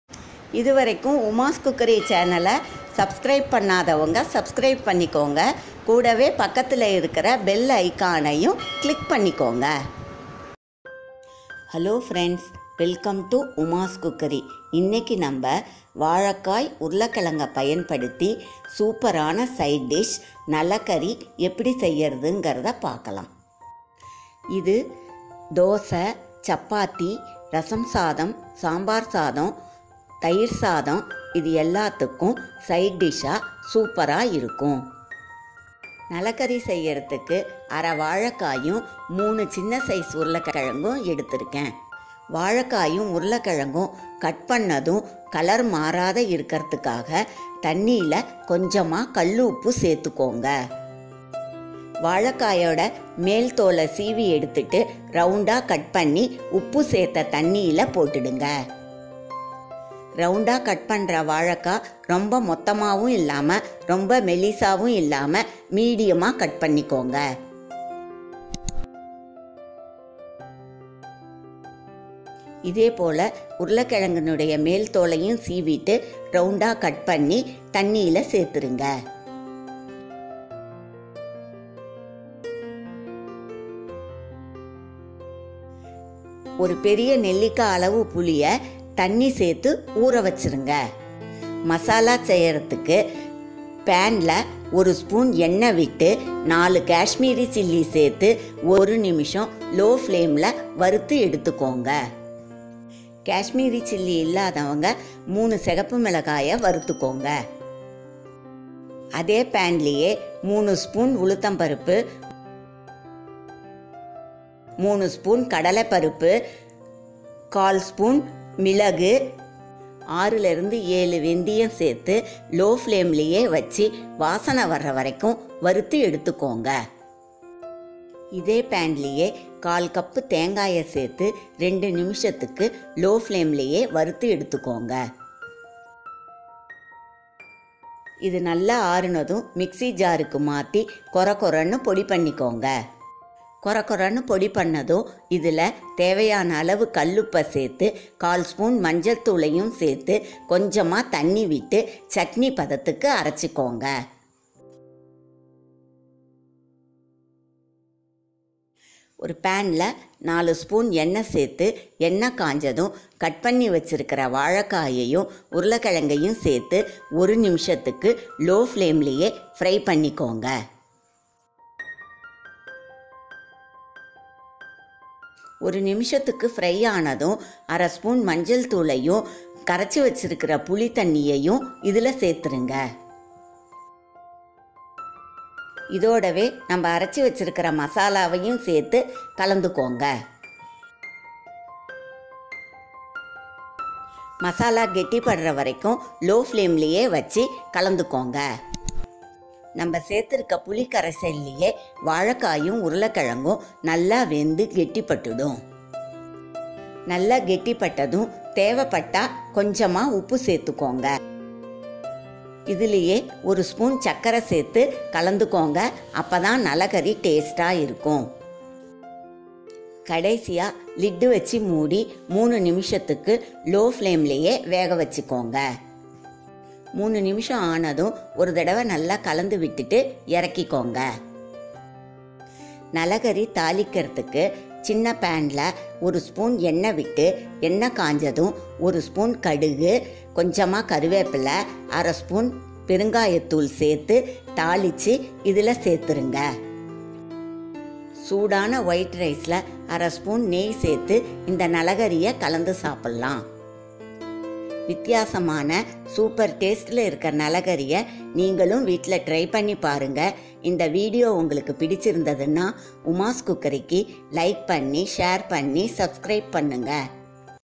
உருளைக்கிழங்கு & வாழைக்காய் இருந்தா இப்படி சூப்பரான காரசாரமான சைடிஷ் செஞ்சி பாருங்க I NALAKARI !!!
How to make Nalakari in tamil

Ingredients

Raw banana                               ½
Potato                                         03 (small size)
Tamarind                                   amla size
Kashmiri chilli                           04
Urad dal                                    03 spn
Chana dal                                  03 spn
Pepper                                       ¼ spn
Fenugreek                                  06 no
Crystal salt                                required
Sugar                                         01 spn